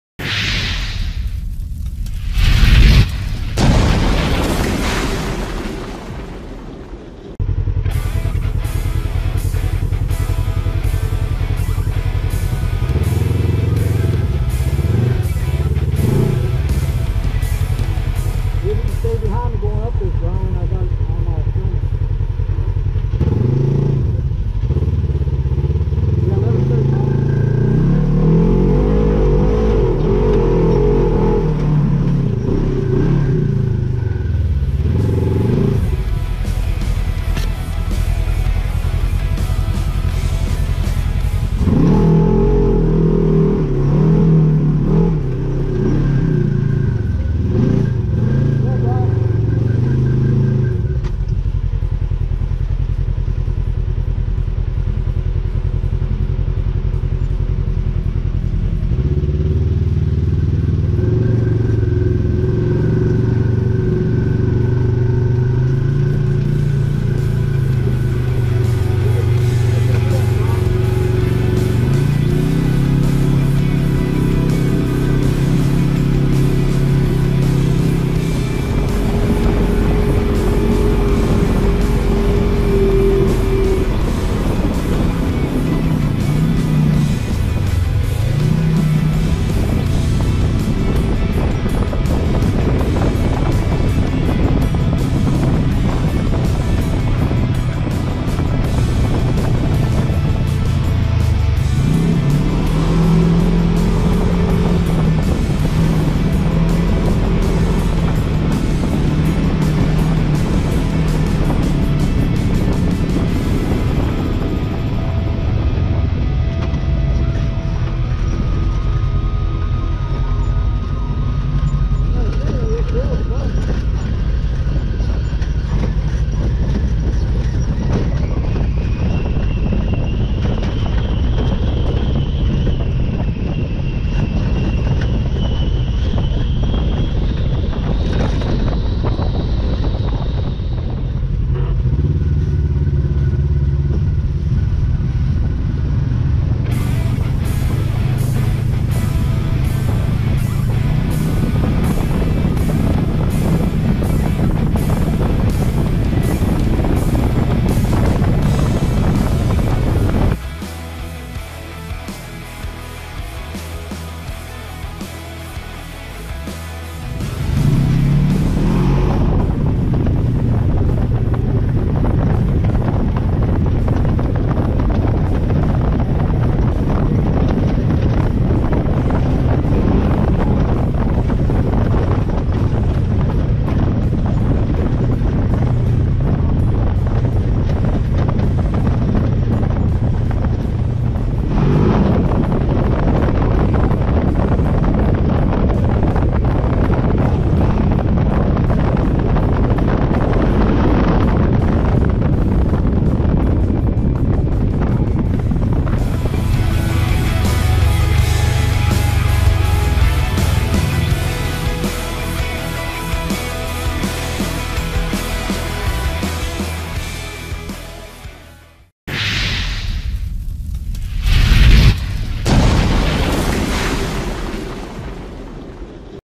Hatfield McCoy Day 1
We rode some outlaws to High rock and the Warrior Trail System. Didn't get to much footage. We did manage to hit all the red and black but of course I didn't have the cameras rolling. Warrior Trails are great